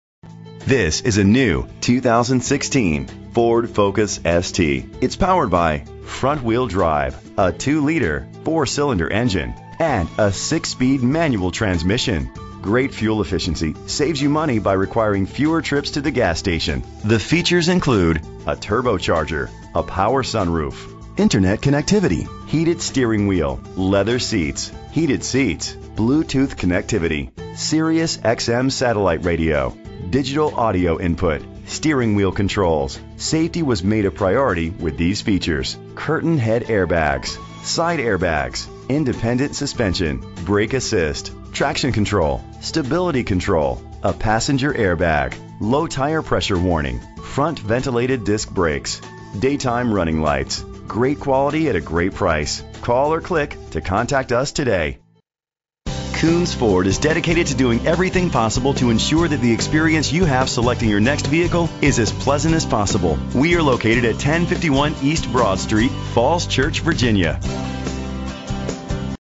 2016 Ford Focus ST - Falls Church VA
Phone:
Year: 2016
Make: Ford
Model: Focus ST
Trim:
Engine: 2.0 liter inline 4 cylinder 16 valve
Transmission: 6-Speed Manual
Color: Shadow Black
Mileage: 0
Address:
1051 East Broad Street